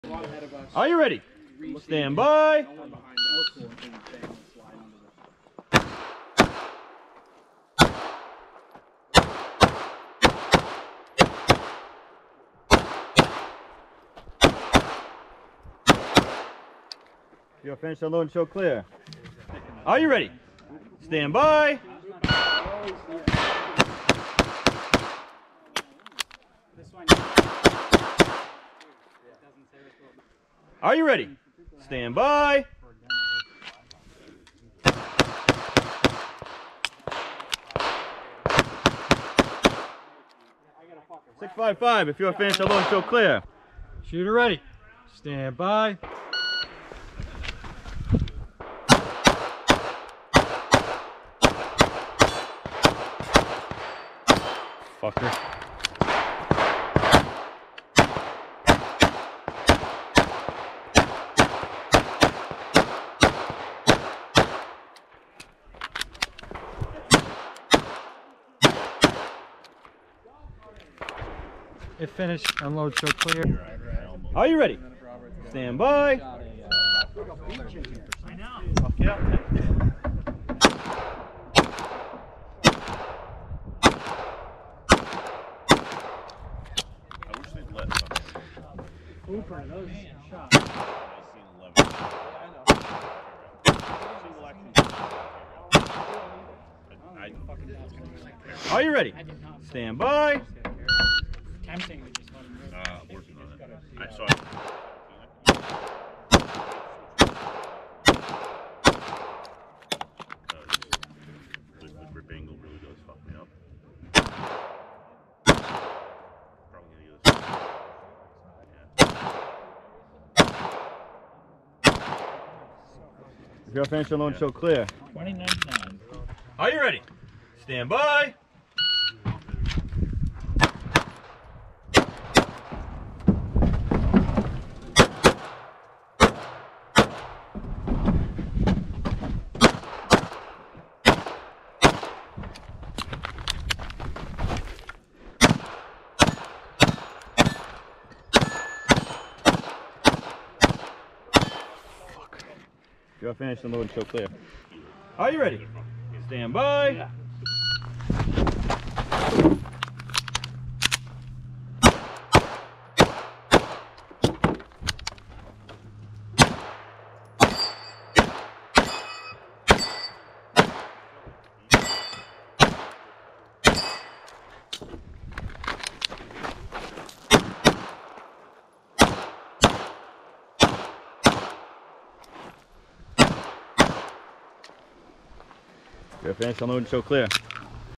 09/19/2020 - SSFGA - USPSA
Sanford-Springvale Fish & Game Protective Association
Berwick, ME

I haven't shot the P07 since I put the CGW pro package in it. I obviously could use more practice with it. I've gotten used to the SRO's larger window.

Firearm used:
Cajunized CZ P07 w/ Trijicon RMR